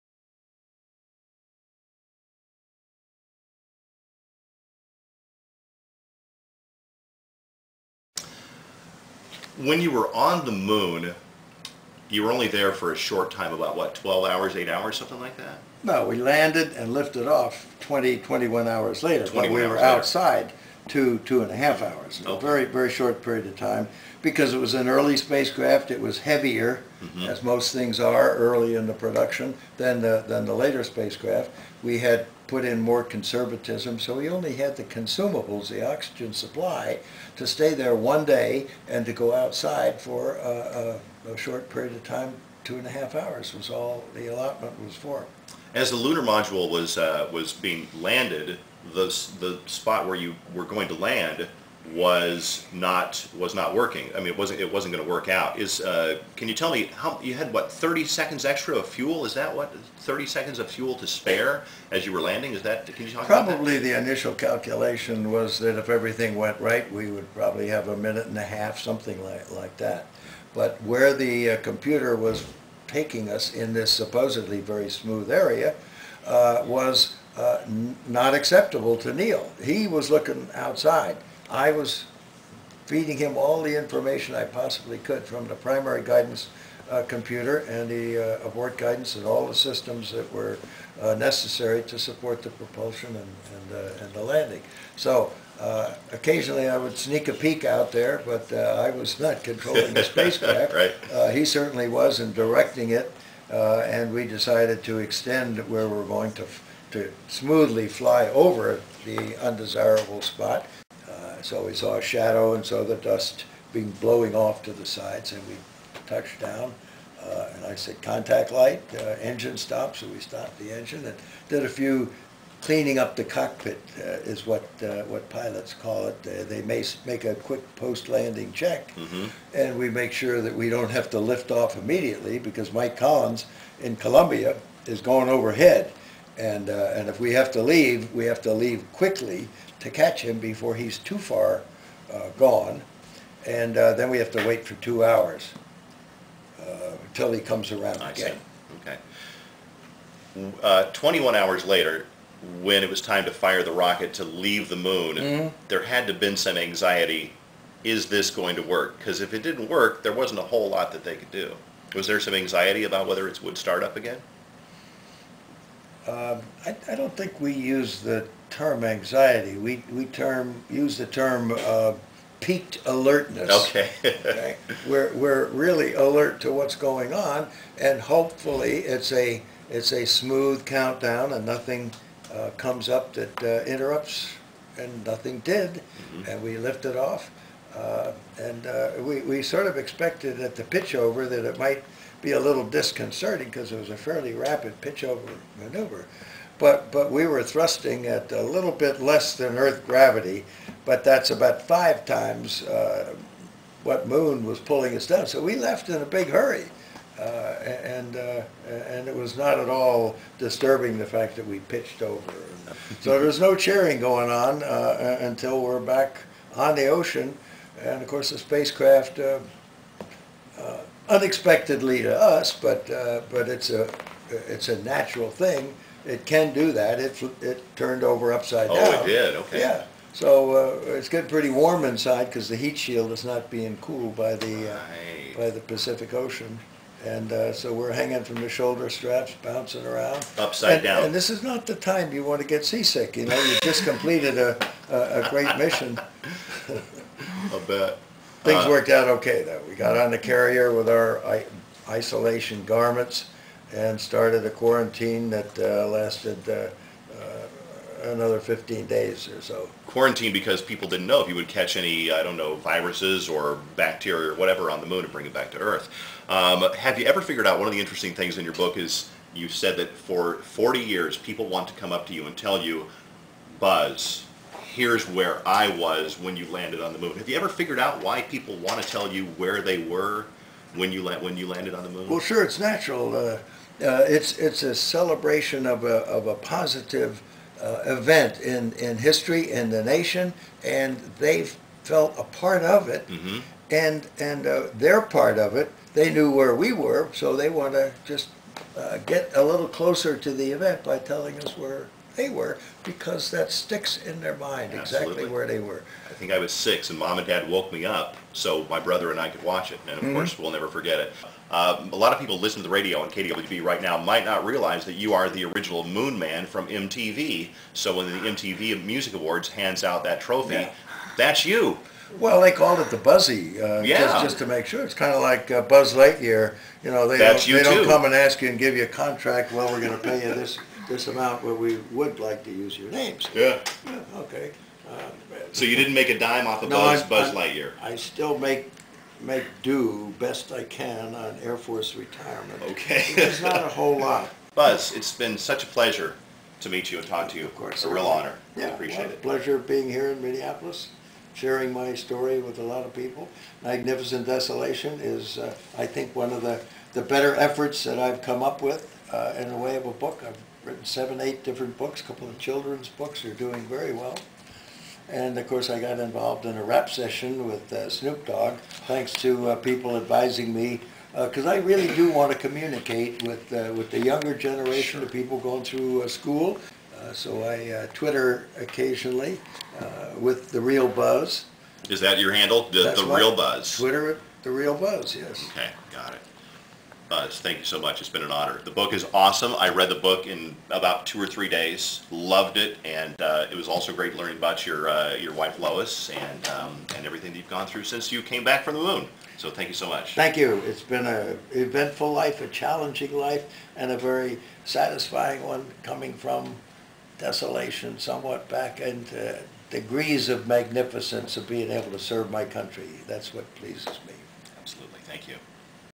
Buzz Aldrin Interview
Dave got to interview astronaut Buzz Aldrin at Mall of America.  Buzz was the second man on the moon, about 15 minutes after Neil Armstrong.